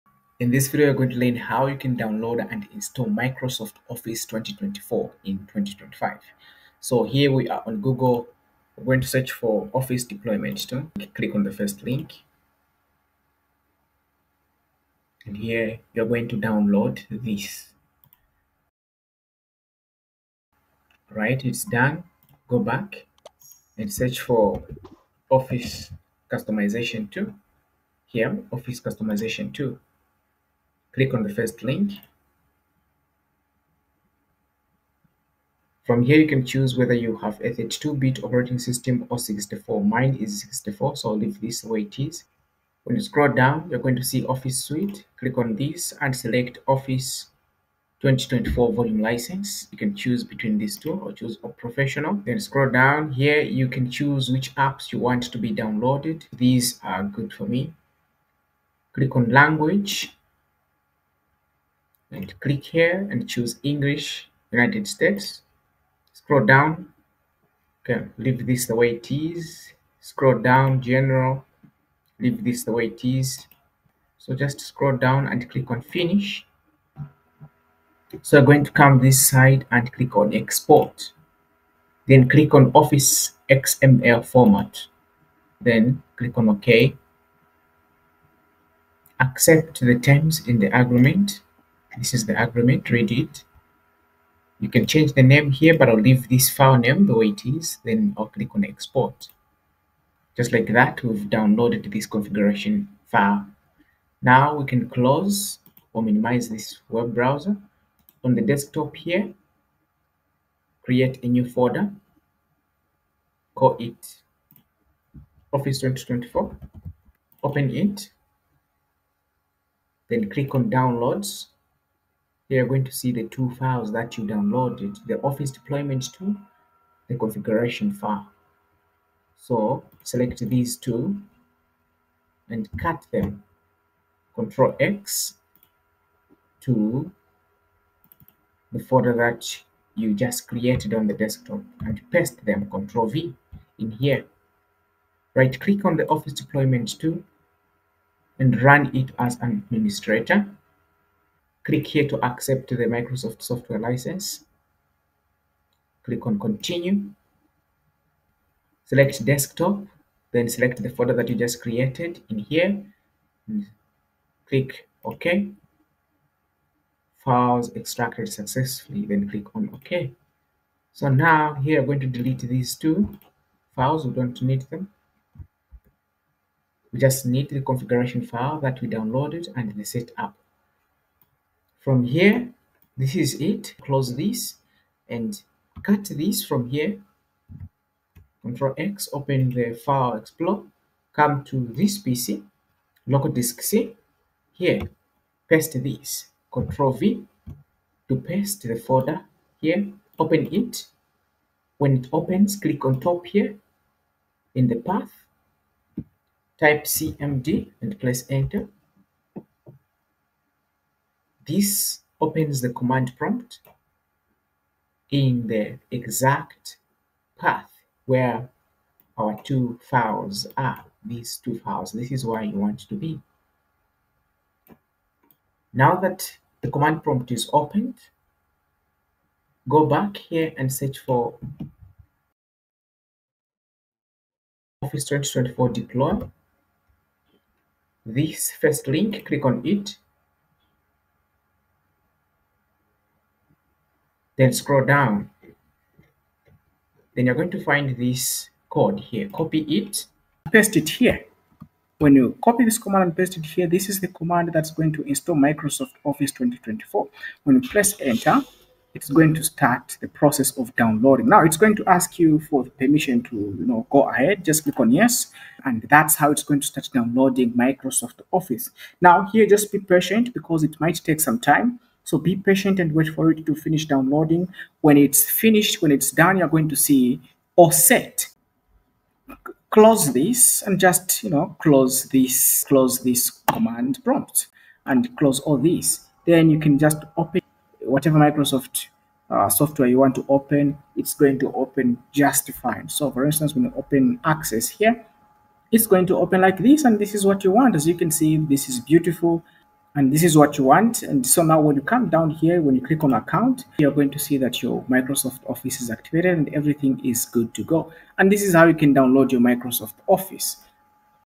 Download and Install Office 2024 From Microsoft for free - Genuine Version
A guide on how to download and install Office 2024 From Microsoft for free a Genuine Version? Check out this video for a quick and easy tutorial on how to download and install Office 2024.

Run into any problems?

🔊 PC Speaker
🖱️ Wireless Mouse
⌨️ Wireless Keyboard and Mouse Combo

🎥 Video Title: download and install office 2024 from Microsoft for free genuine version office 2024 activation